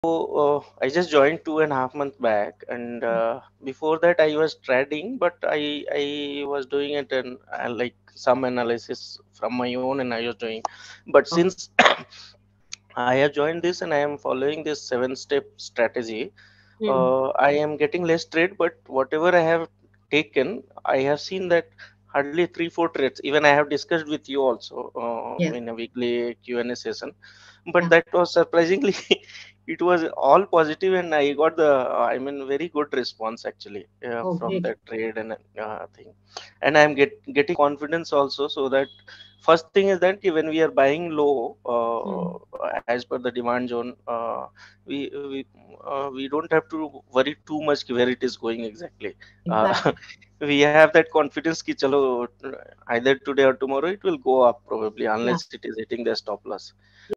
Learn Price Action Trading Reviews | 7-Step Price Action Strategy | LPAT Edge Pro Program
Gulam Ahmad, one of our LPAT Edge Pro Prgoram, shares his experience learning the 7-Step Simple Rule-based Price Action Strategy and How he has increased hi confidence and Success Rate.